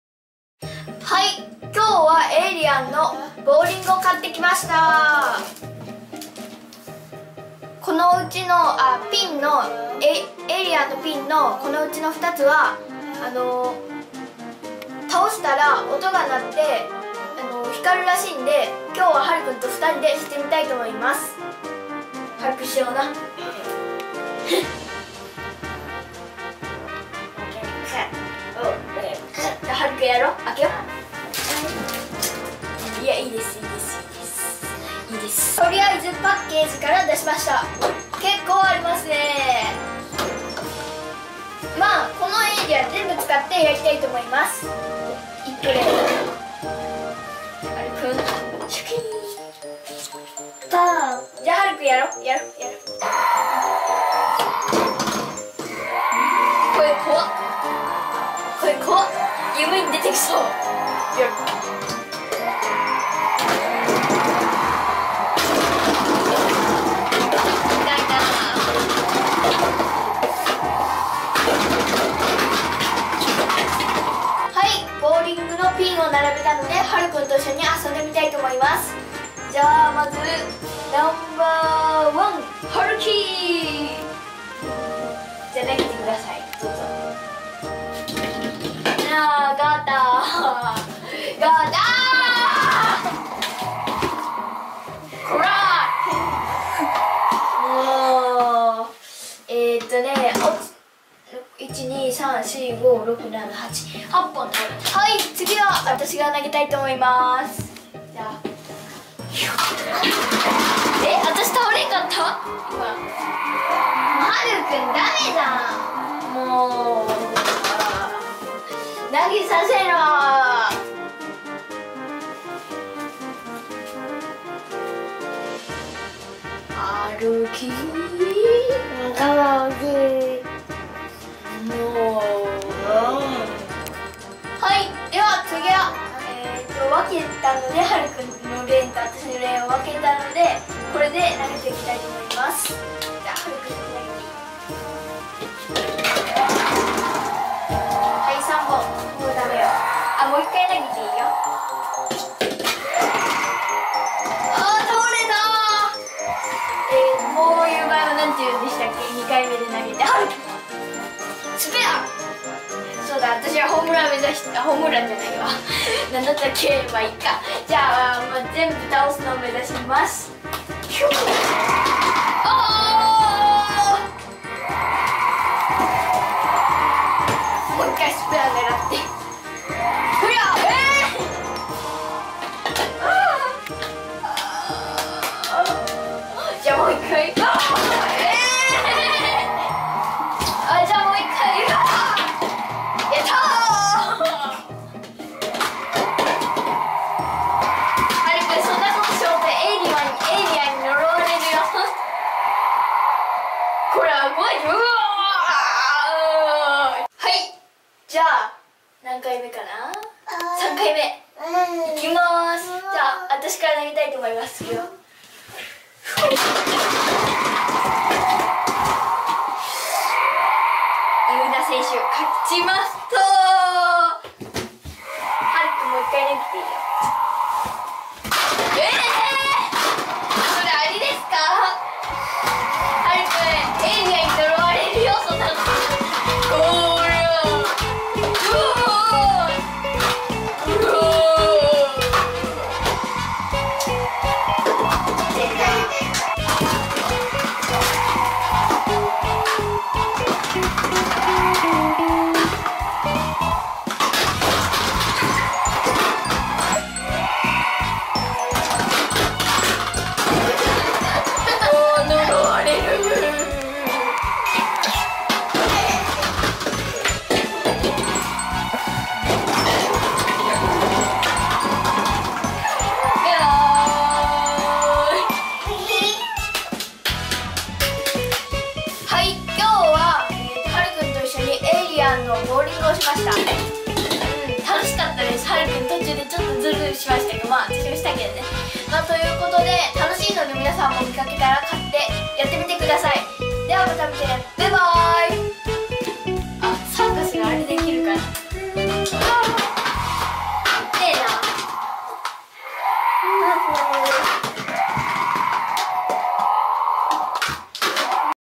エイリアン ボーリングで遊びました。I played alien bowling
エイリアンボーリングを２人でしました。